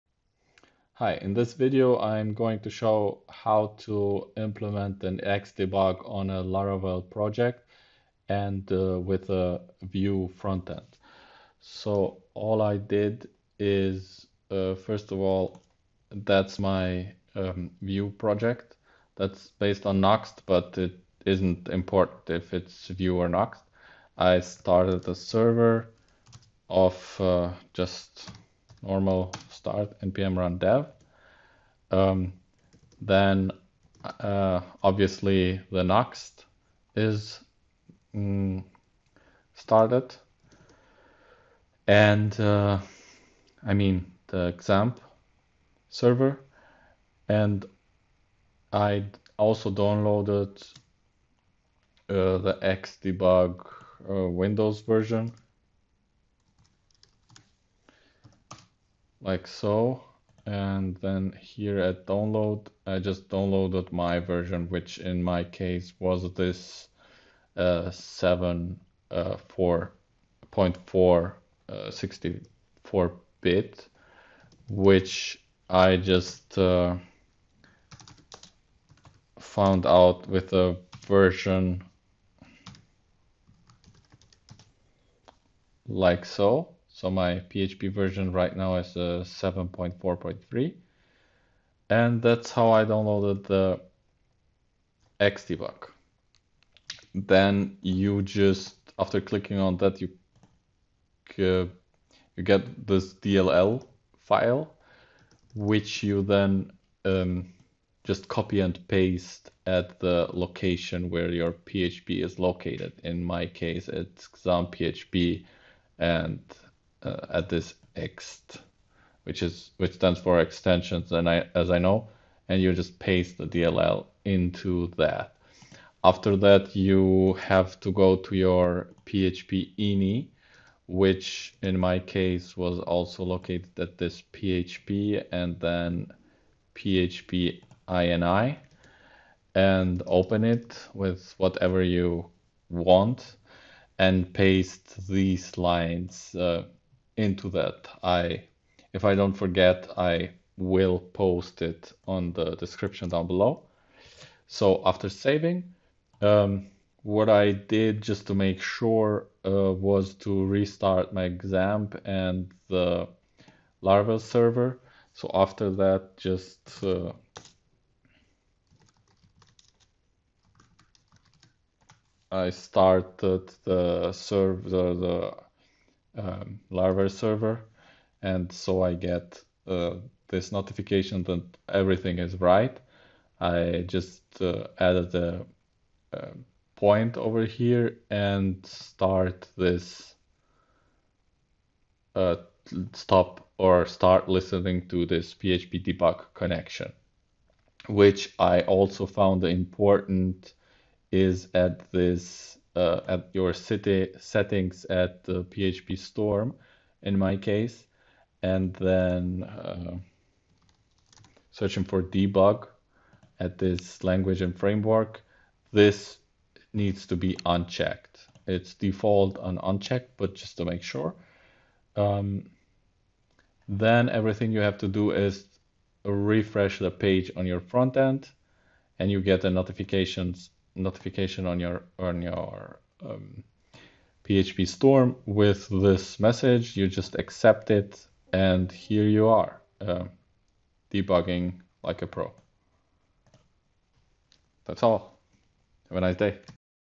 Install xdebug on Phpstorm, for Laravel environment and vue.js (js frameworks) on frontend
In this video I am showing how to install xdebug on a phpstorm environment with a laravel project on the backend and vue.js on the frontend but the frontend is not that important it could be anything react or angular are also easy to do.

php.ini config code:

[Xdebug]
zend_extension = "c:\xampp\php\ext\php_xdebug.dll"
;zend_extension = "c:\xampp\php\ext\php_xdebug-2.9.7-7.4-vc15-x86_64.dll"
xdebug.remote_autostart = 1
xdebug.profiler_append = 0
xdebug.profiler_enable = 0
xdebug.profiler_enable_trigger = 0
xdebug.profiler_output_dir = "c:\xampp\tmp"
;xdebug.profiler_output_name = "cachegrind.out.%t-%s"
xdebug.remote_enable = 1
xdebug.remote_handler = "dbgp"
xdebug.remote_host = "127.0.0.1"
xdebug.remote_log = "c:\xampp\tmp\xdebug.txt"
xdebug.remote_port = 9000
xdebug.trace_output_dir = "c:\xampp\tmp"
;36000 = 10h
xdebug.remote_cookie_expire_time = 36000